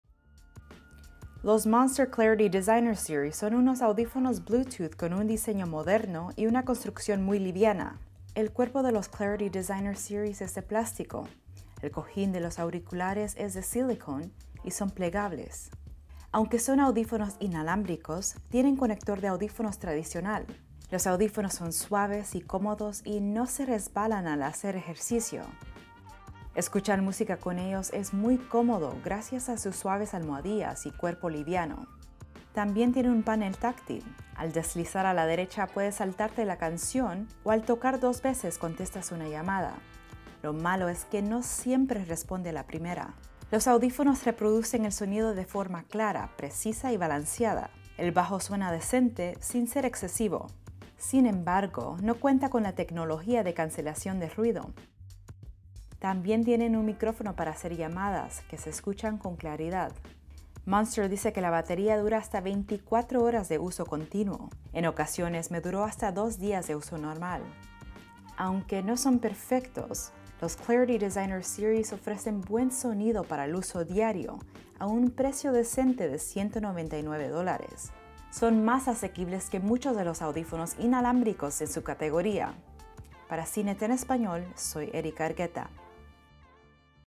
Monster Clarity BT Designer Series: audífonos bluetooth ligeros y con batería duradera
Los Monster Clarity BT Designer Series son audífonos bluetooth ligeros y con una batería que dura hasta 24 horas de uso continuo.